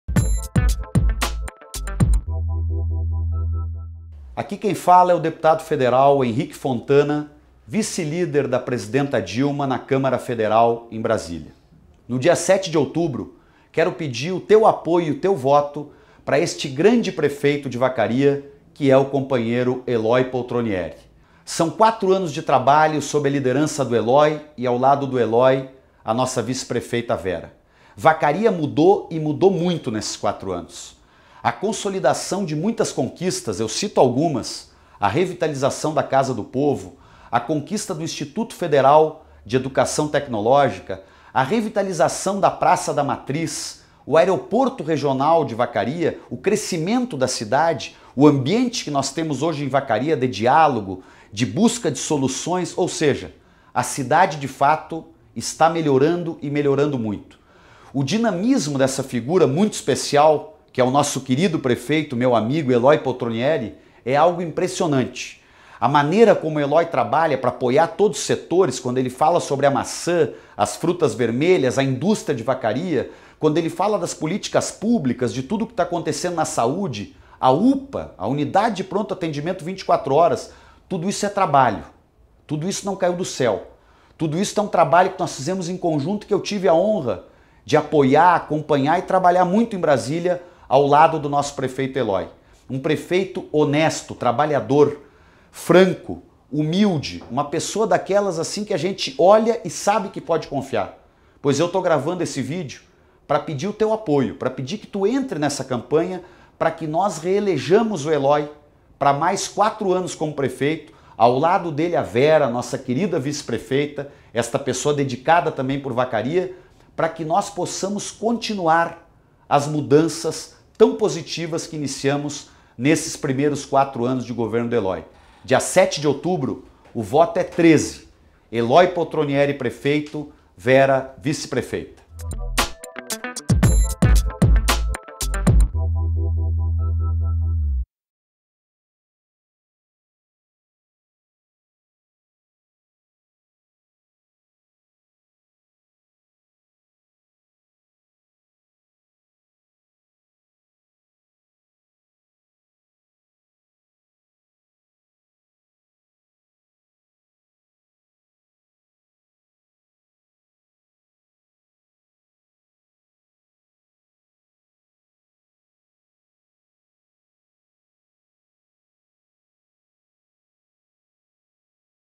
Vacaria é PT: Deputado Henrique Fontana apoia Elói
O deputado federal, Henrique Fontana (PT-RS) declarou apoio ao candidato Elói Poltronieri (PT) para prefeito do município de Vacaria, no estado do Rio Grande do Sul. "São quatro anos de trabalho sobre a liderança do Elói e ao lado do Elói a nossa vice-prefeita Vera. Vacaria mudou muito nesses últimos anos e com a consolidação de muitas conquistas como, por exemplo, a revitalização da Casa do Povo, a conquista do Instituto Federal de Educação Tecnológica, a revitalização da Praça da Matriz, o Aeroporto Regional de Vacaria, o crescimento da cidade e o ambiente que nós temos hoje em Vacaria de diálogo e de busca de soluções, ou seja, a cidade de fato esta melhorando e melhorando muito. Então no dia sete de outubro o voto é 13 com Elói Poltronieri para prefeito e a Vera como vice-prefeita" explicou.

Fabricia Neves - Portal do PT